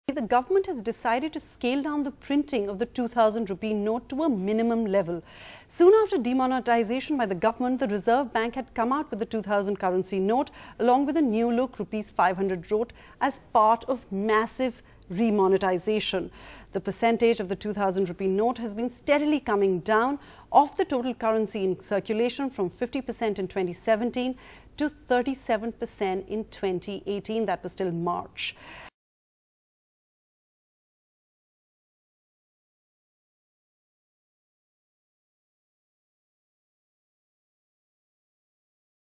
Rs. 2,000 Note Printing Scaled Down To Minimum By RBI: Report
The printing of Rs. 2,000 notes, which debuted after Prime Minister Narendra Modi's shock demonetisation move in 2016, has been reduced to the "minimum" by the Reserve Bank of India (RBI), a top finance ministry official said today amid reports that it is being phased out.